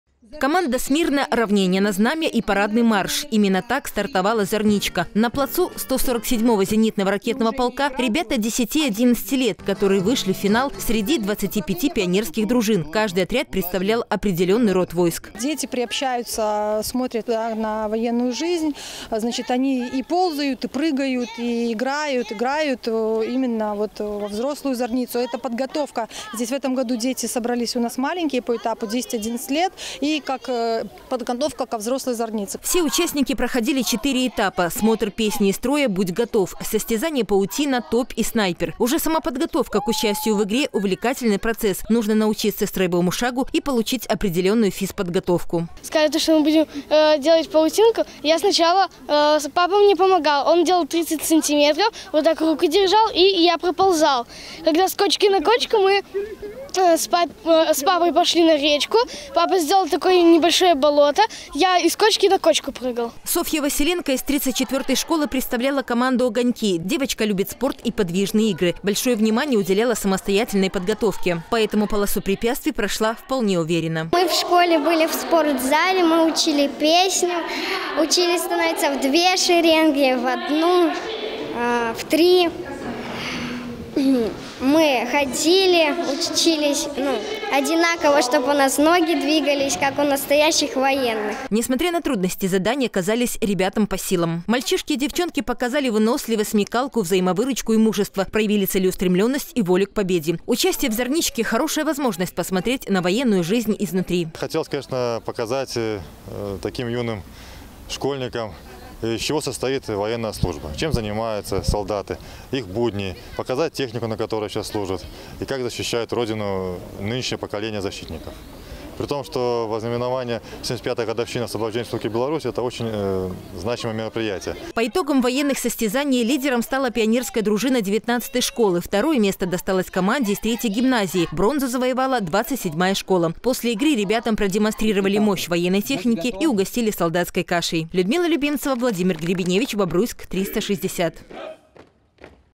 В стиле милитари!
Эстафеты, викторины и знакомство с уставом Вооруженных Сил Беларуси. На базе 147-го зенитного ракетного полка прошел финал городской спортивно-патриотической игры.
Эта «Зарничка» приурочена к 75-летию освобождения Беларуси от немецко-фашистских захватчиков и собрала более 100 участников.